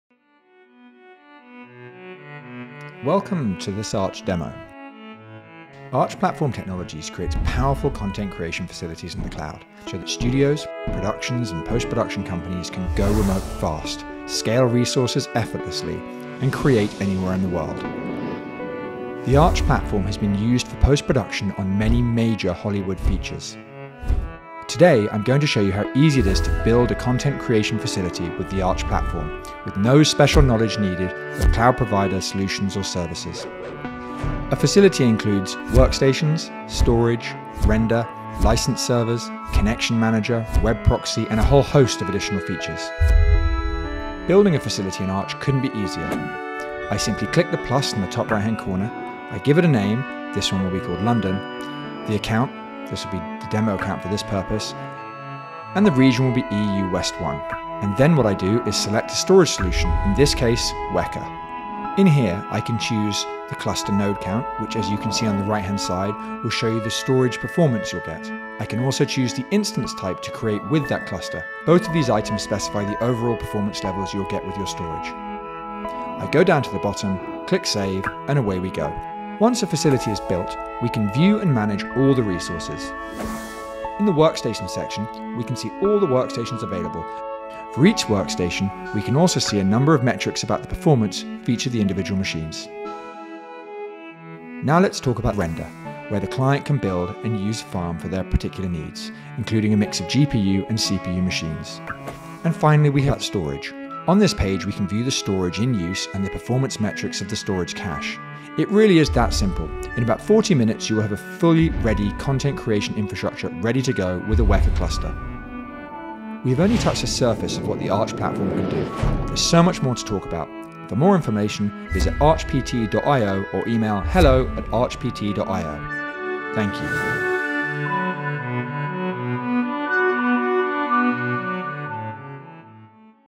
WekaIO + Arch Technologies Platform: Building a VFX Studio In the Cloud in Under 45 Minutes
WekaIO and Arch Platform Technologies has teamed up to offer WekaFS, the fastest multi-protocol compliant file system on the AWS cloud. WekaFS delivers very high streaming bandwidth to the most demanding applications such as Autodesk Flame, while maintaining low latency between the workstations and the storage system. WekaFS has demonstrated it has the ability to main performance and keep up with high frame rates and high bit-depth workflows. The following customer commented sums up the power of Weka’s high performance storage system on AWS.

WekaFS offers a multi-protocol POSIX-compliant file system with full file shareability and file locking across different operating systems. Windows, Mac, and Linux users can access and share a single project set allowing for easy collaboration among creative teams.

Arch Platform Technologies provides the entertainment industry’s only full cloud resource orchestration platform for visual effects, post-production, and virtual production. Arch has built out cloud resources for studios and post-production companies alike – from boutique studios born in the cloud to major VFX facilities with hundreds of user seats.

-- ►ADDITIONAL RESOURCES👇
Solution Brief: Cloud-Based Post-Production Infrastructures with WekaIO and Amazon Web Services
Build a digital studio-in-the-cloud with Amazon Nimble Studio and WekaIO